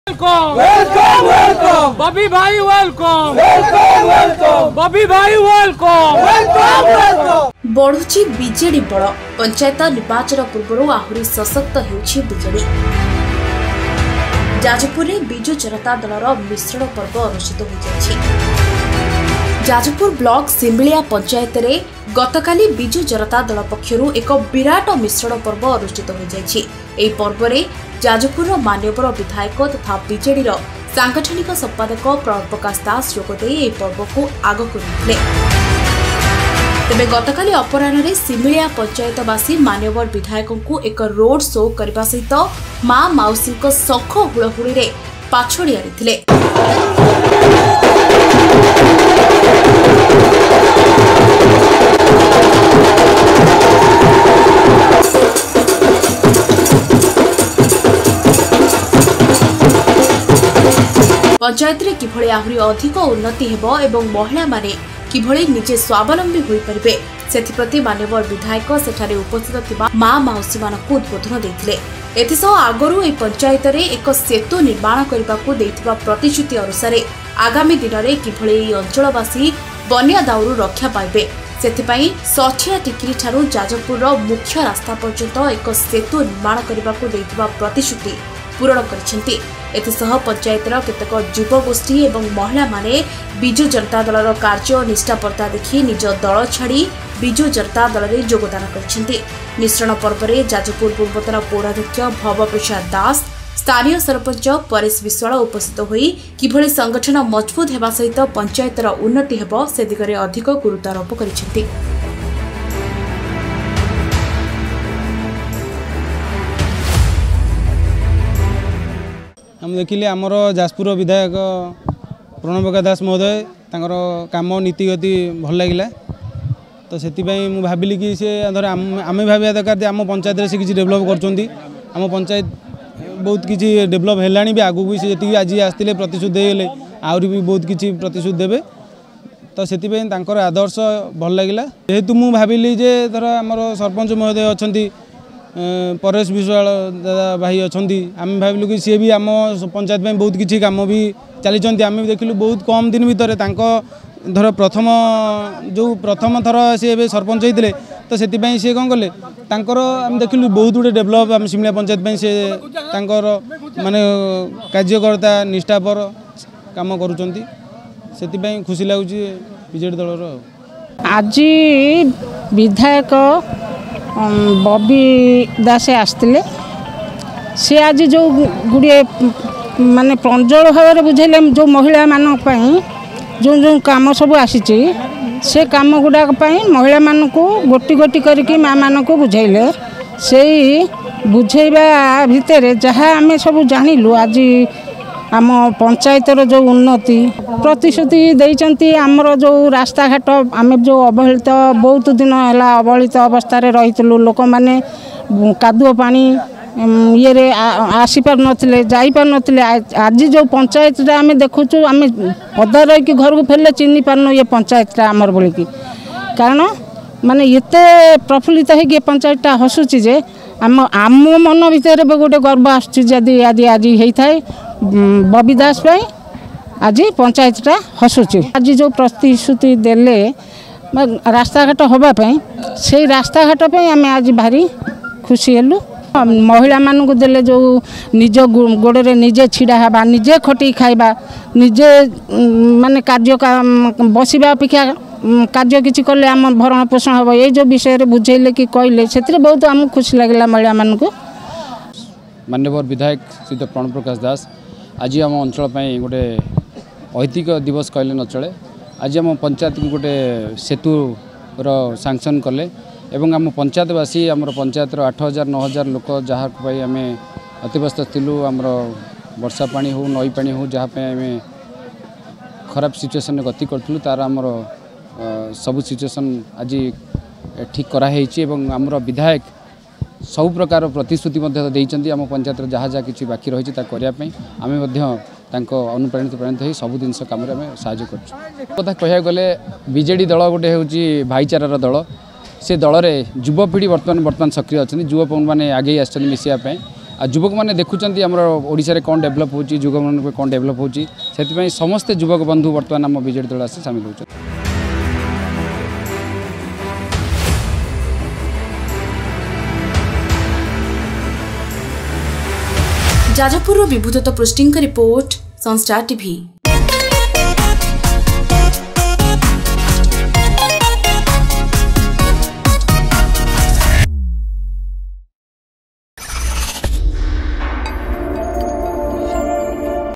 ବଢୁଛି ବିଜେଡି ବଳ, ପଞ୍ଚାୟତ ନିର୍ବାଚନ ପୂର୍ବରୁ ଆହୁରି ସଶକ୍ତ ହେଉଛି ବିଜେଡି ବିଜେଡିର ମହା ମିଶ୍ରଣ ପର୍ବ ଅନୁଷ୍ଠିତ
ବଢୁଛି ବିଜେଡି ବଳ । ପଞ୍ଚାୟତ ନିର୍ବାଚନ ପୂର୍ବରୁ ଆହୁରି ସଶକ୍ତ ହେଉଛି ବିଜେଡି । ଯାଜପୁରରେ ଆଜି ବିଜୁ ଜନତା ଦଳର ମିଶ୍ରଣ ପର୍ବ ଅନୁଷ୍ଠିତ ହୋଇଛି । ସିମିଳିଆ ପଞ୍ଚାୟତର ବହୁ ଯୁବକ ମହିଳା ବିଜୁ ଜନତା ଦଳରେ ଯୋଗ ଦେଇଛନ୍ତି । ଯାଜପୁର ବିଧାୟକ ତଥା ବିଜେଡିର ସାଙ୍ଗଠନିକ ସମ୍ପାଦକ ପ୍ରଣବ ପ୍ରକାଶ ଦାସ ମିଶ୍ରଣ ପର୍ବରେ ଯୋଗଦେଇ କର୍ମୀଙ୍କୁ ଉତ୍ସାହିତ କରିଛନ୍ତି